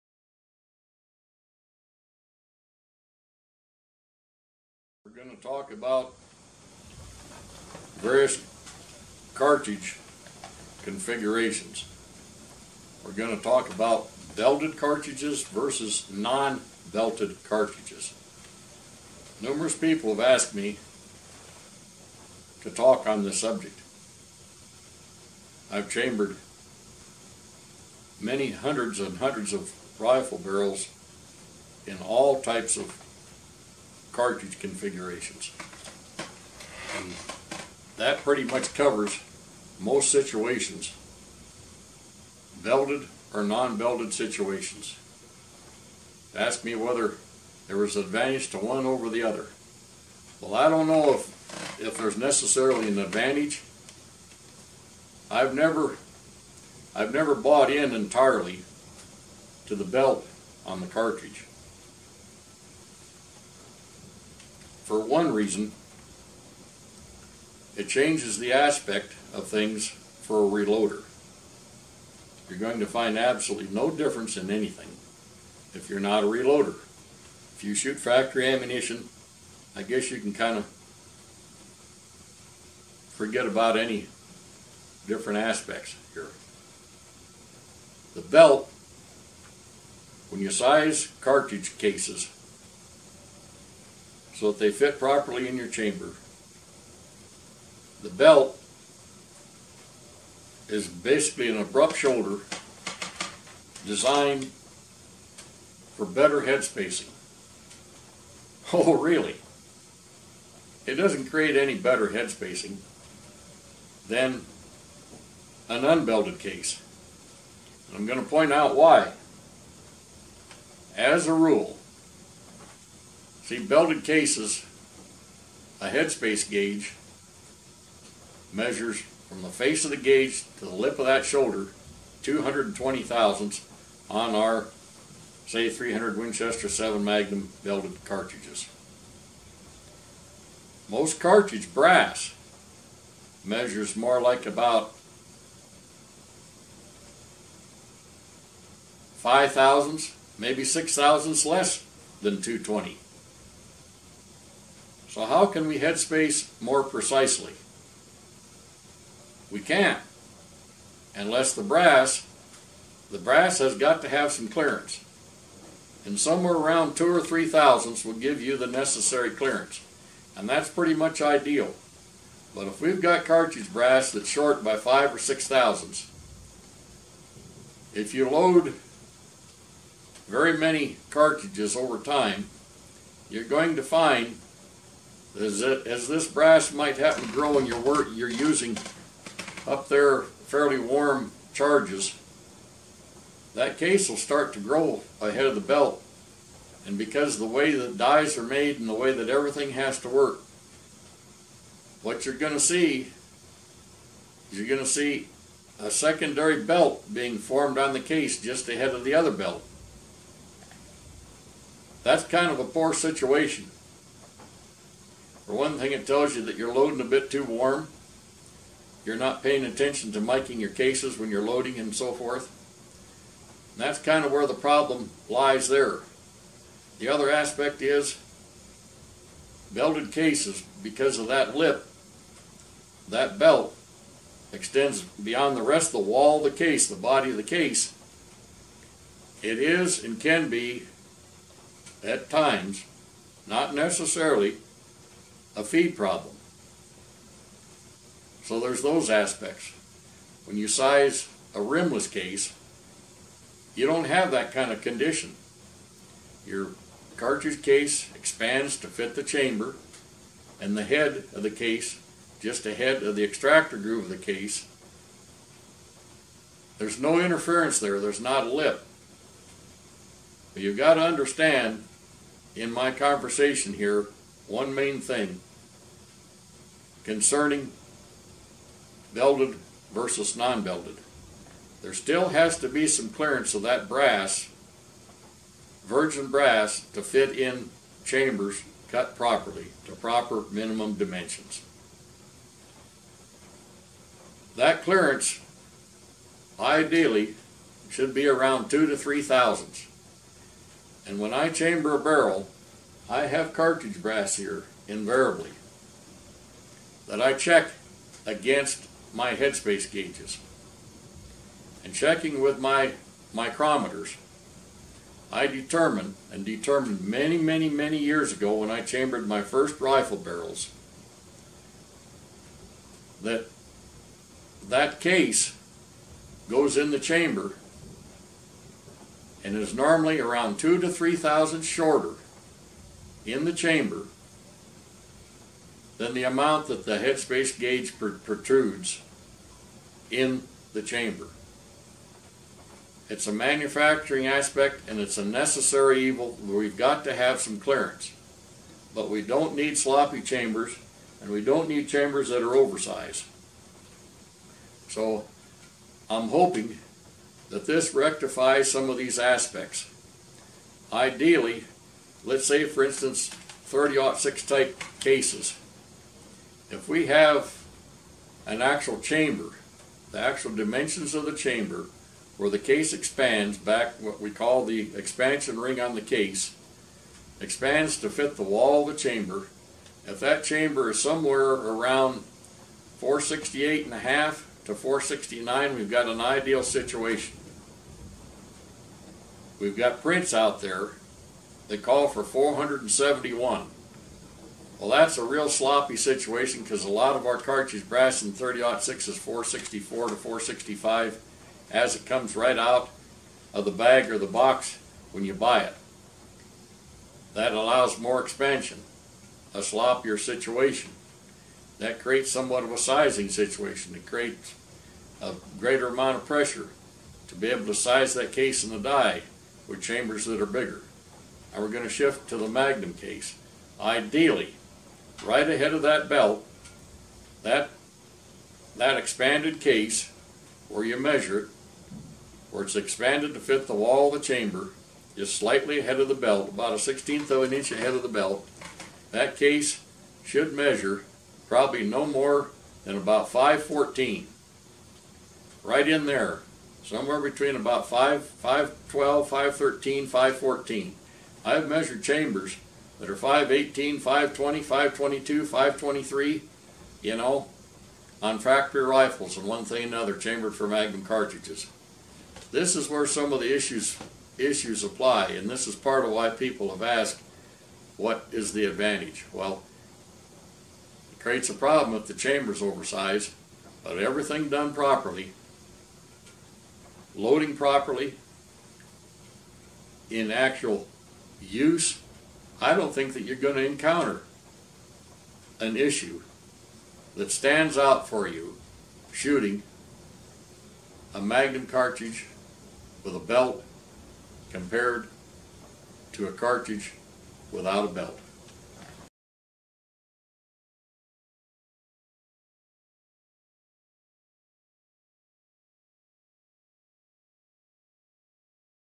Belted vs Unbelted Cartridges
Randy discusses the differences between belted and unbelted cartridges and why the difference ma be important if you are a reloader.